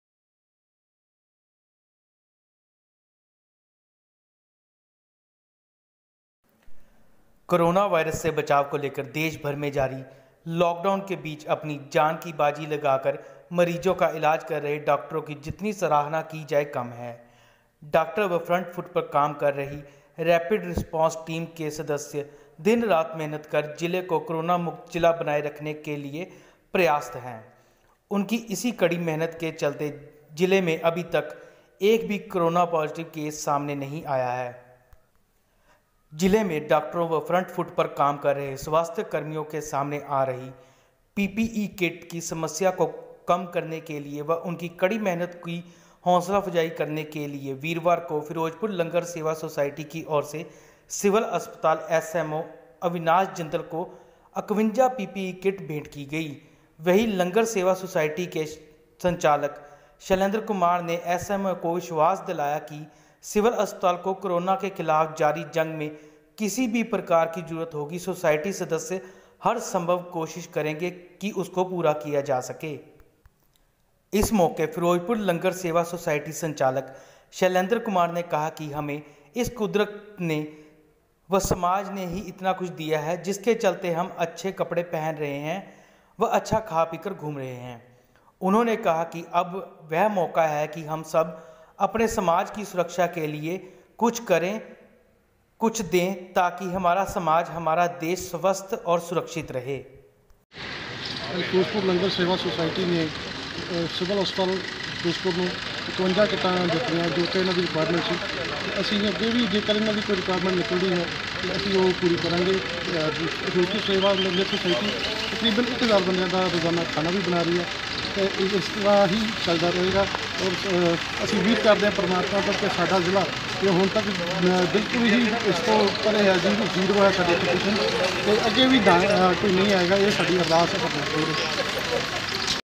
COVID-19 : 51 PPE KITS DONATED TO CIVIL HOSPITAL FEROZEPUR, WATCH
कोरोना वायरस से बचाव को लेकर देशभर में जारी लॉकडाउन के बीच अपनी जान की बाजी लगाकर मरीजों का इलाज कर रहे डाक्टरों की जितना सराहना की जाए कम है । डाक्टर व फ्रंट फुट पर काम कर रही रैपिड रिस्पोंस टीम के सदस्य दिन-रात मेहनत कर जिले को कोरोना मुक्त जिला बनाए रखने के लिए प्रयासरत हैं । उनकी इसी कड़ी मेहनत के चलते जिले में अभी तक एक भी कोरोना पॉजिटव केस सामने नहीं आया है । जिले में डाक्टरों व फ्रंट फुट पर काम कर रहे स्वास्थ्य कर्मियों के सामने आ रही पीपीई किट की समस्या को कम करने के लिए व उनकी कड़ी मेहनत की हौंसलाफजाई के लिए वीरवार को फिरोजपुर लंगर सेवा सोसायटी की ओर से सिविल अस्पताल एसएमओ अविनाश जिंदल को 51 पीपीई किट भेंट की गई । वहीं लंगर सेवा सोसायटी के संचालक शैलेंद्र कुमार ने एसएमओ को विश्वास दिलाया कि सिविल अस्पताल को कोरोना के खिलाफ जारी जंग में किसी भी प्रकार की जरूरत होगी सोसायटी सदस्य हर संभव कोशिश करेंगें की उसे पूरा किया जा सके ।
इस मौके फिरोजपुर लंगर सेवा सोसायटी संचालक शैलेंद्र कुमार ने कहा कि हमें इस कुदरत ने व समाज ने ही इतना कुछ दिया है जिसके चलते हम अच्छे कपड़े पहन रहें व अच्छा खा पीकर घूम रहे हैं । उन्होंने कहा कि अब वह मौका है कि हम अपने समाज की सुरक्षा के लिए कुछ करें समाज को कुछ दें ताकि हमारा समाज हमारा देश स्वस्थ व सुरक्षित रहे ।

The doctors who are treating patients by putting their lives in the midst of a lockdown across the country to protect against the corona virus are less appreciated. Doctors and members of the Rapid Response Team working on the front foot are working day and night to make the district a corona-free district. Due to this hard work, not a single Corona positive case has been reported in the district so far. 51 PPE to Civil Hospital SMO Avinash Jindal on behalf of Ferozepur Langar Service Society on Thursday, to reduce the problem of PPE kit facing doctors and health workers working on front foot in the district. Kit presented. On the other hand, Shailendra Kumar, the Director of the Anchor Service Society, assured the SMO that the civil hospital will need any kind in the ongoing war against Corona, the Society members will do everything possible to fulfill it.
On this occasion, Ferozepur Langar Service Society Director Shailendra Kumar said that this nature and society has given so much to us, due to which we are walking around wearing good clothes and drinking good food. He said that now is the chance that we do something for the protection of our society and give something to the society so that our society will be healthy and safe.